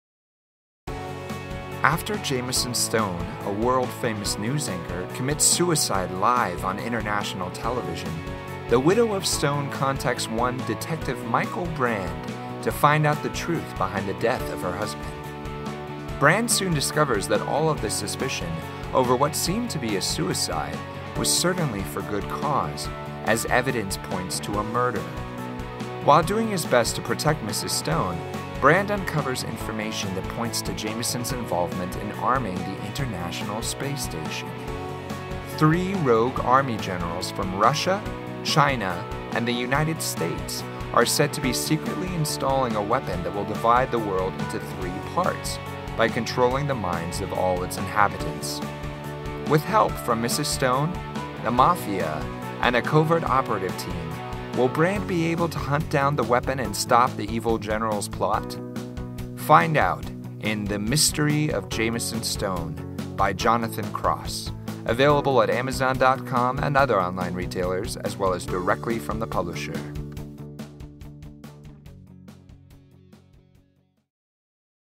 The Mystery of Jamieson Stone by Jonathan Cross
When Jamieson Stone, the world's most famous television news anchor, apparently commits suicide on live international television, Stone's widow hires detective Michael Brand to find out the truth. Brand discovers that Stone was murdered, but the "how and why" behind the crime is a mystery. While doing his best to protect Mrs. Stone, Brand uncovers information that points to Jamieson's involvement in arming the international space station.